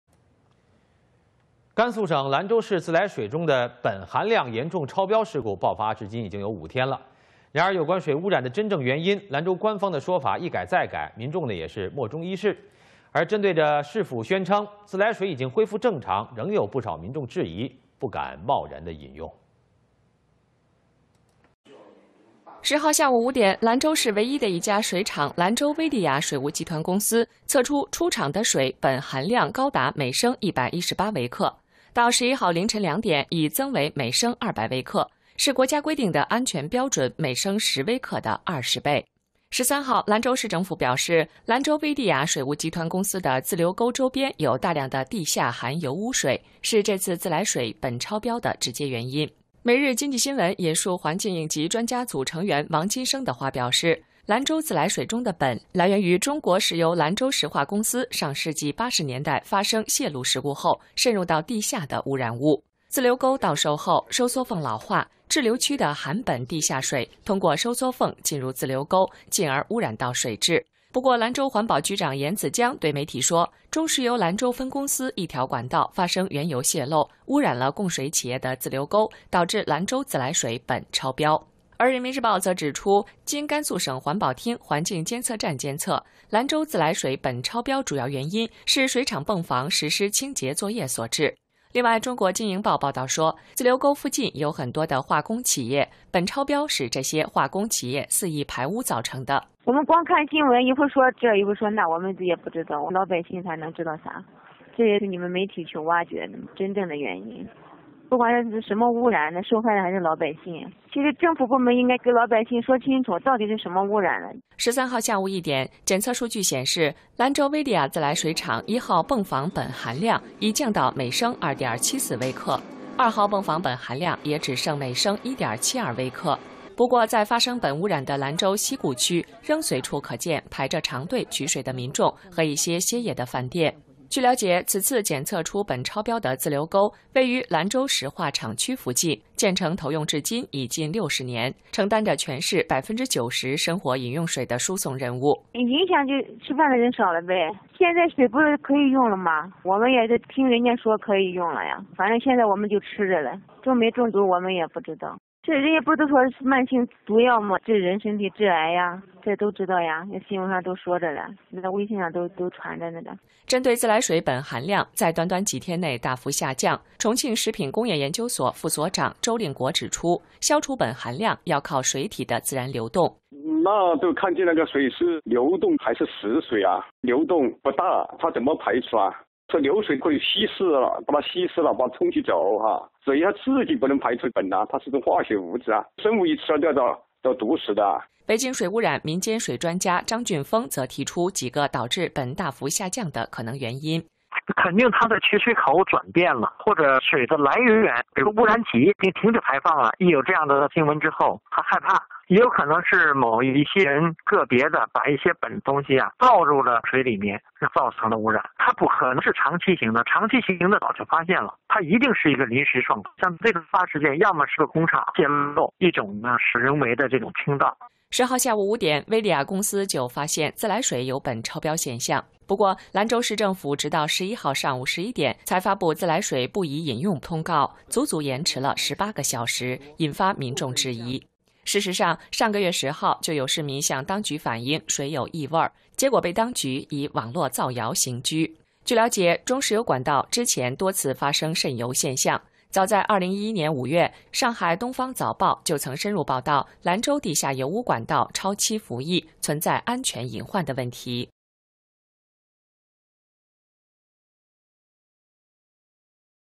Lanzhou Drinking Water Polluted with Excessive Benzene
For five days, excessive benzene has been detected in Lanzhou's
drinking water.
However, officials have not given a confirmed explanation
for the pollution.
People hesitate to use the water even though the official claims
the water is clear of all pollutant.

At 17:00 on March 10, Lanzhou water plant of Lanzhou
Veolia Water Company,
found 118 micrograms of benzene per liter of water and 200
micrograms per liter at 2:00 on March 11.
This makes it 20 times the national safety standard,
with 10 micrograms of benzene per liter of water.

Lanzhou municipal government said, the benzene came
from oily wastewater.

According to Wang Jinsheng, local environment task force
member, Lanzhou Petrochemical Co is to blame for the pollution.
The oil leak in the 80s seeped into the ground and has re-emerged
as the aging of retention to the area, polluting the water sources.

However, Lanzhou environmental protection bureau
Yan Zijiang said,
that the oil has leaked through a pipeline of Lanzhou
Petrochemical Co,
contaminating the water supply with
excessive benzene.

The People's Daily reported that Gansu Provincial Environmental
Protection Bureau,
has identified the water plant pump room cleaning operation,
resulted in the contamination of the water supply.

In addition, China Business said the arbitrary discharges from
chemical companies surrounding the gravity flow
channels, are the cause of excessive benzene.

A Lanzhou restaurant clerk: "We don't know the exact cause of
the pollution. The media needs to find out for us.
We are the victims. The government should explain to us."

At 13:00 of April 13, benzene levels at Lanzhou Veolia Water Plant
at pumping station one,
had dropped to 2.74 micrograms per liter, and 1.72 micrograms
per liter at pumping station two.
However, local residents are still lining up to pick up water.
Restaurants are also closed due to the water problem.

It is said that the gravity flow channel with excessive benzene is
located near the Lanzhou Petrochemical plant.
This particular channel has supported 90 percent of all drinking water
in Lanzhou for nearly 60 years.

Restaurant clerk: "It affects business, a lot less people will
come to the restaurant.
Now we just use the water no matter if it is poisonous or not,
the government said it is ok.
According to a news report and WeChat, It was said there is a chronic
and cancer causing poison in the water."

Regarding the significant drop of benzene levels in the
last few days,
Chongqing Institute of Food Industry deputy director
Zhou Lingguo pointed out,
benzene elimination depends on the
water's natural flow.

Zhou Lingguo, Chongqing Institute of Food Industry deputy
director: "The flowing water can dilute it, but the
near static water can not.
The water itself can't discharge it.
It is a chemical which kills."

Beijing water pollution expert Zhang Junfeng explains some
possible reasons for the sharpl decrease in benzene levels.

Zhang Junfeng, Beijing water pollution expert: "Surely they
have changed the water source or stopped using the polluted sources.
Maybe someone has poured the pollutants in to the water recently.
I don't think its been there long very long;
otherwise it would have been found long ago.
It's just temporary or an incident resulting from a plant leak
or personal dumping."

Veolia water company found excessive benzene in the water
At 17:00 of April 10.
However, Lanzhou municipal government did not issue the
warning of not to use the water until 11:00 am of April 11.
That was a delay of 18 hours.

People questioned a smell in the water was reported
on March 10.
The authorities did nothing but arrest the person who complained
about the smell on charges of 'Internet rumors'.

It is said there were many oil leaks in the Petrochemical
oil pipelines.
On May 2011, Shanghai Oriental Morning Post, reported
about hidden risks of the aged Lanzhou underground oil pipelines.